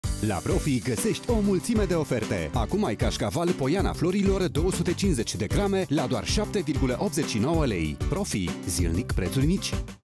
La Profi găsești o mulțime de oferte! Acum ai Cașcaval Poiana Florilor 250g la un pret special.
La Profi găsești o mulțime de oferte! Acum ai Cașcaval Poiana Florilor 250g la un pret special. Oferta supusa unor termene si conditii. Profi. Zilnic preturi mici.